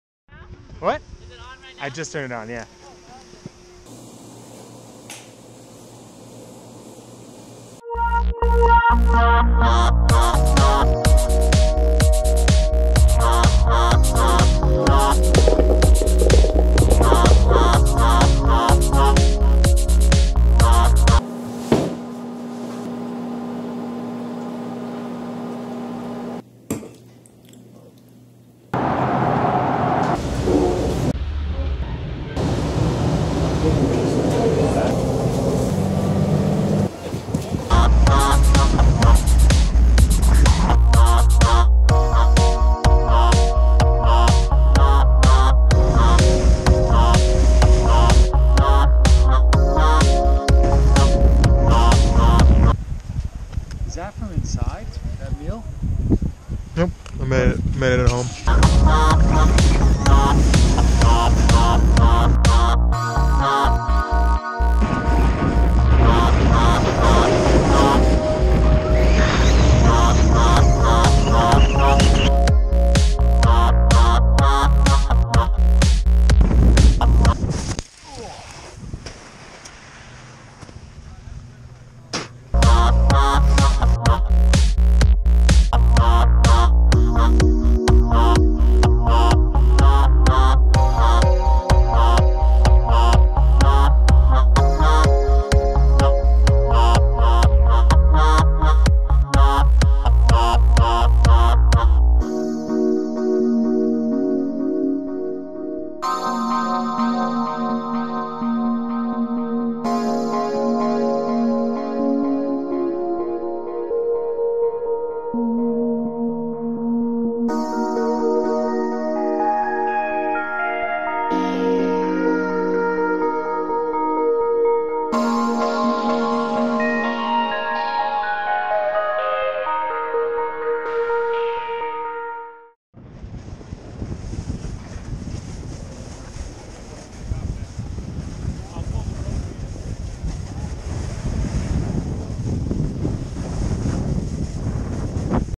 GoPro SESSION Test
Decided to get a GoPro Session. Decided to use it at Wachusett. Good choice.